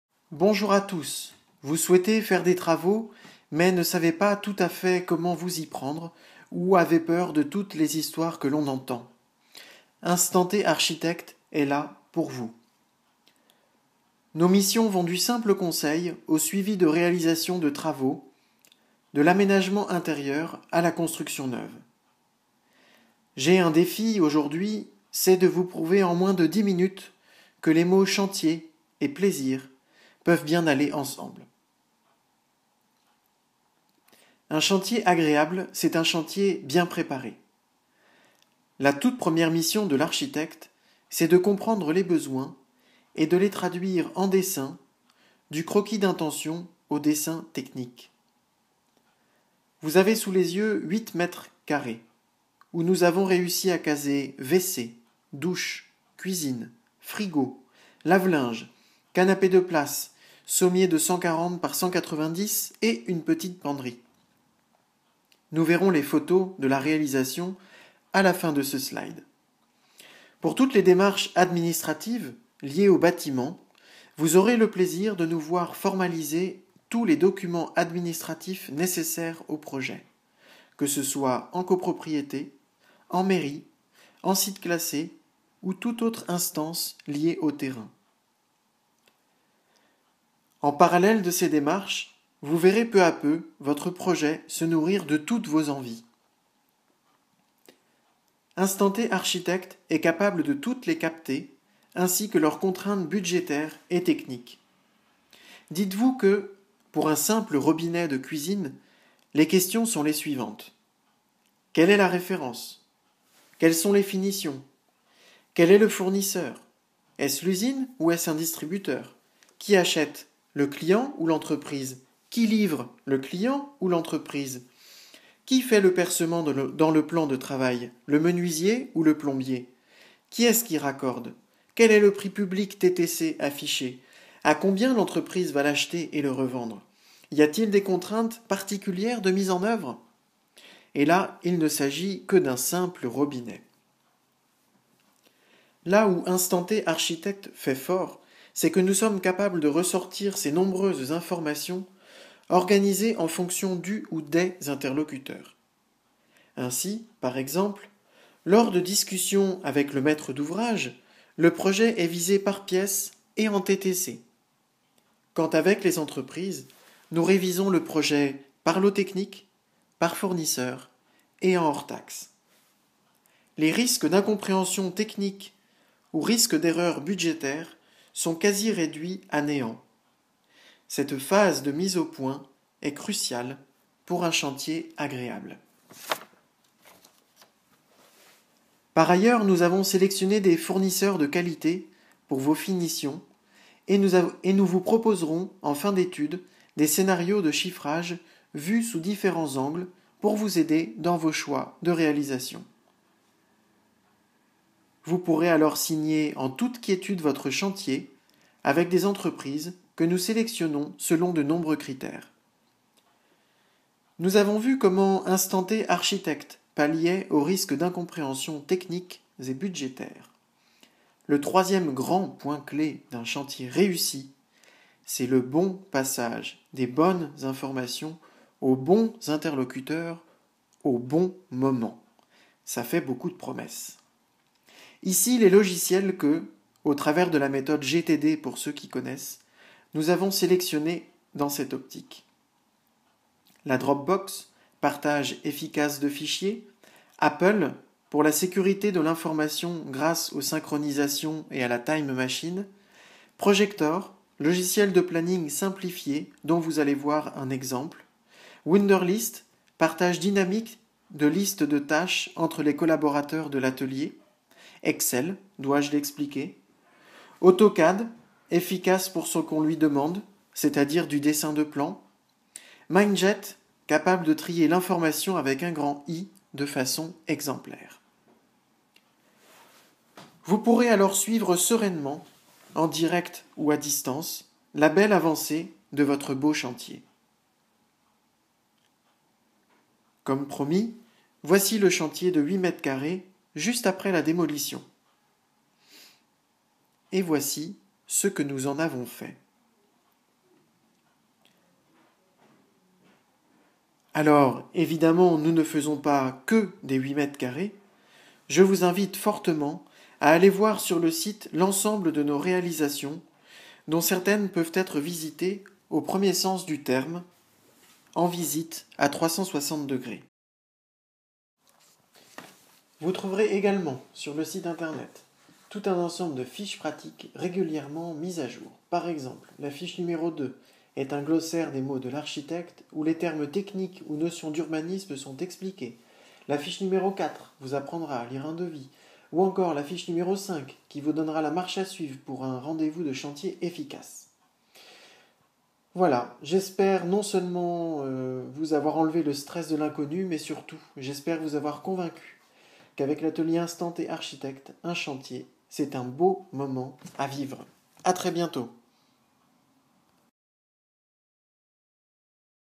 Un chantier nommé Plaisir
Adrien Soviche vous donne les grandes lignes de la prestation d'architecte et de maîtrise d'oeuvre.
L'acte de construire ou de rénover fait partie d'un monde à part, avec un langage à part et des codes à part.
Adrien vous les fait découvrir, et vous révèle comment instant|t y répond et se veut l'interprète entre les professionnels et les particuliers.